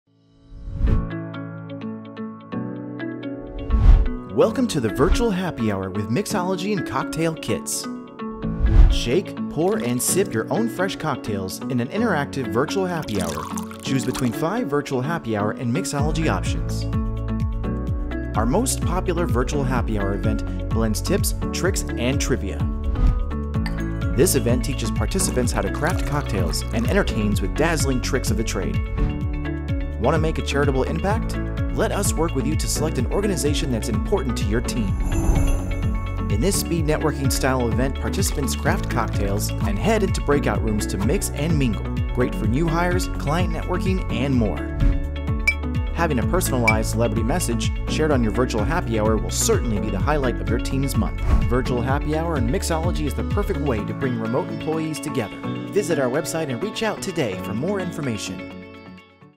Virtual Happy Hour & Mixology | Virtual Programs | TeamBonding
Virtual Happy Hour & Mixology is the perfect way to bring remote employees together and create a sense of camaraderie in the virtual workplace. You can’t go for drinks after work. But having cocktails together hosted by our world-class mixologist by video chat can help fill the void.